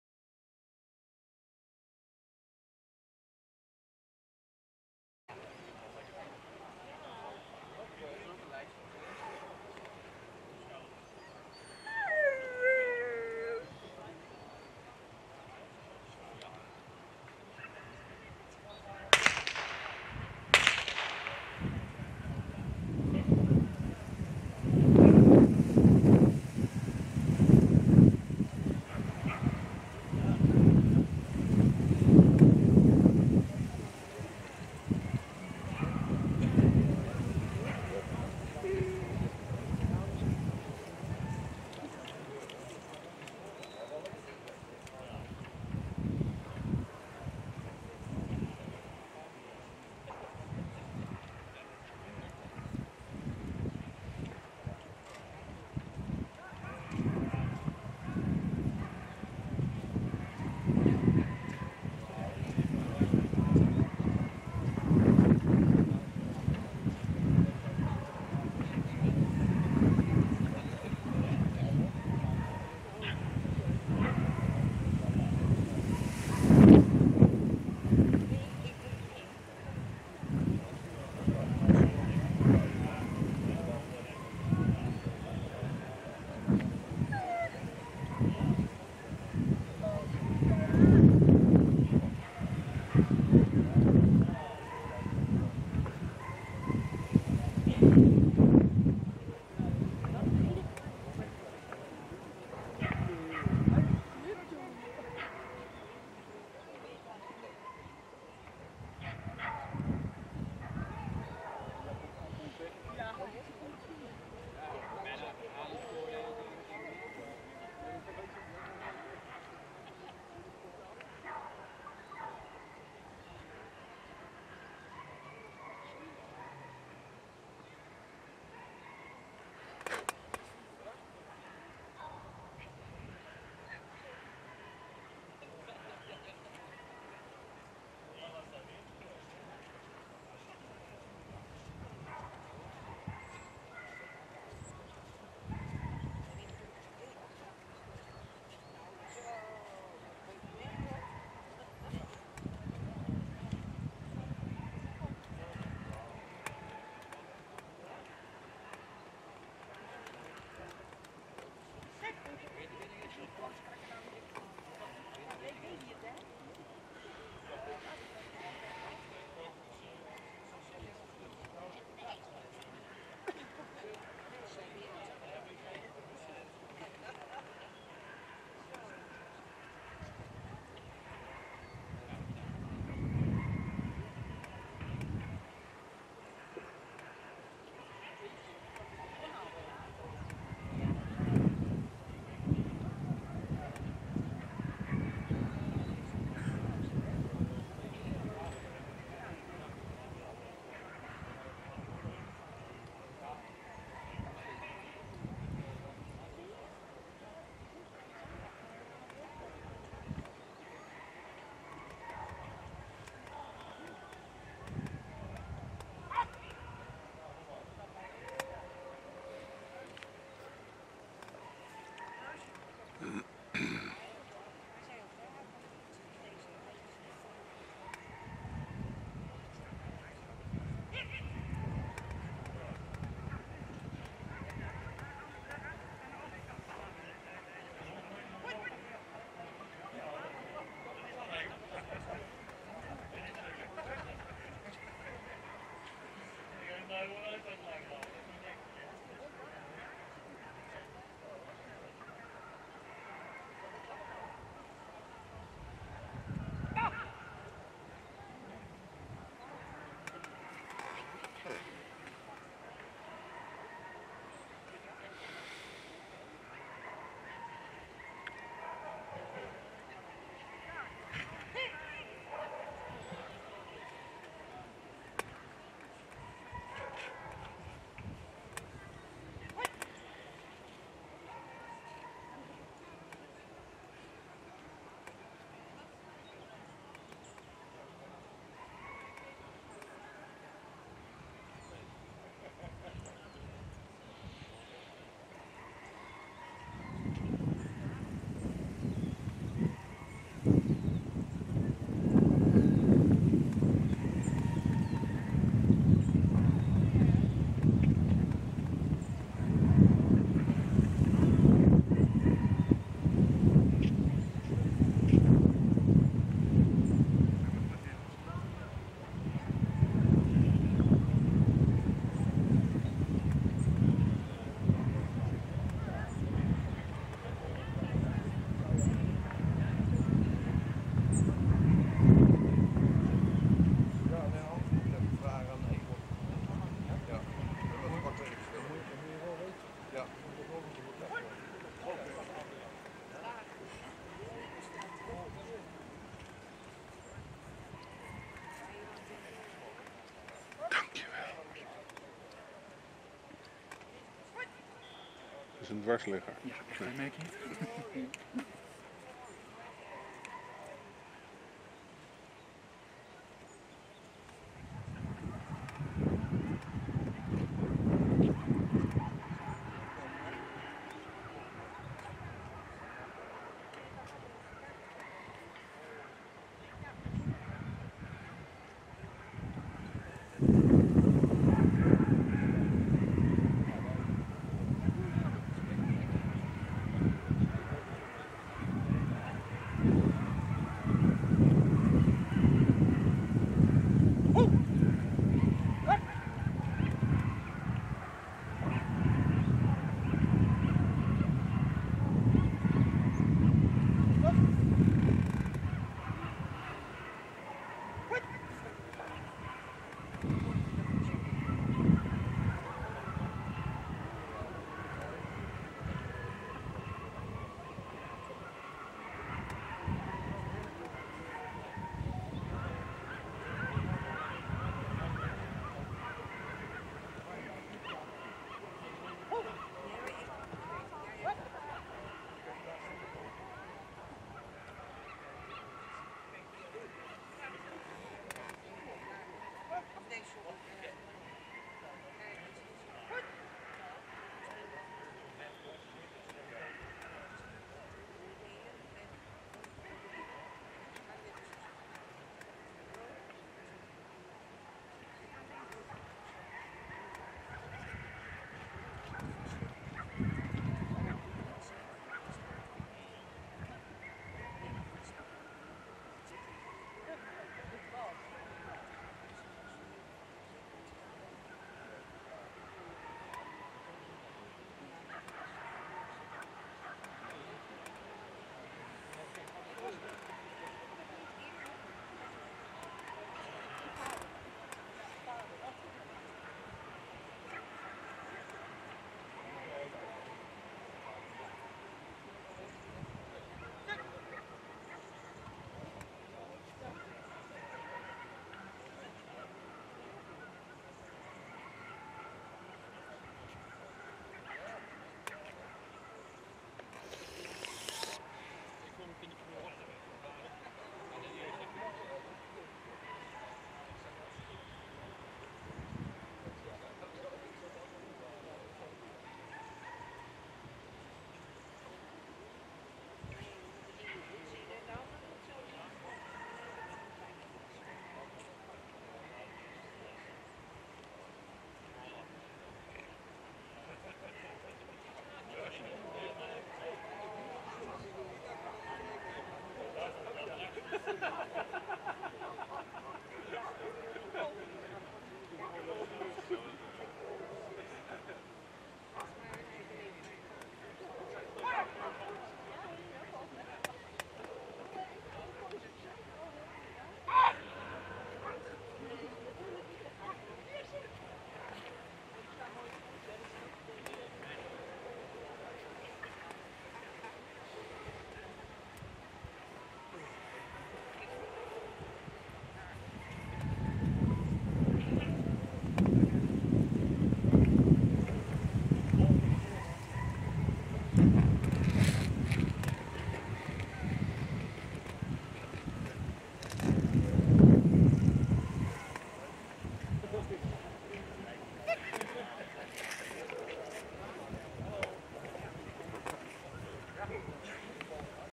Dutch Championship Belgianshepherd Sico Goedhart Blöf vom puren Leben Obedience
Dutch Championship Belgianshepherd Sico Goedhart Blöf vom puren Leben Obedience 89P